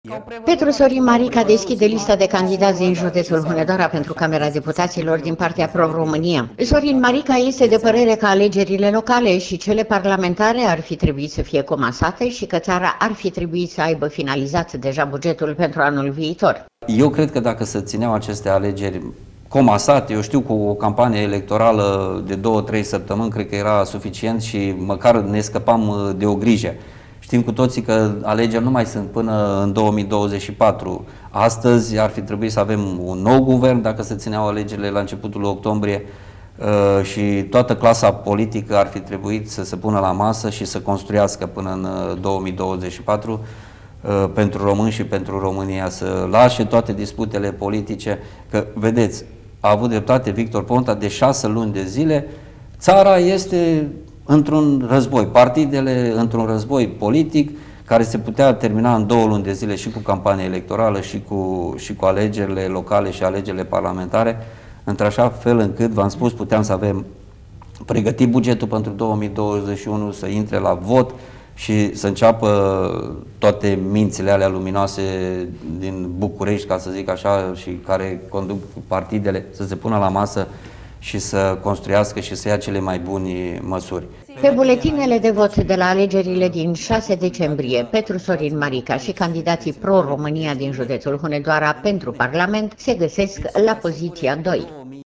PETRU SORIN MARICA (PRO ROMÂNIA): “AR FI TREBUIT SĂ AVEM DEJA BUGETUL PE 2021”
Petru Sorin Marica deschide lista de candidați din județul Hunedoara pentru Camera Deputaților din partea Pro România. Candidatul Pro România pentru Camera Deputaților, Petru Sorin Marica, este de părere că alegerile locale și cele parlamentare ar fi trebuit să fie comasate și că țara ar fi trebuit să aibă finalizat deja bugetul pentru anul viitor.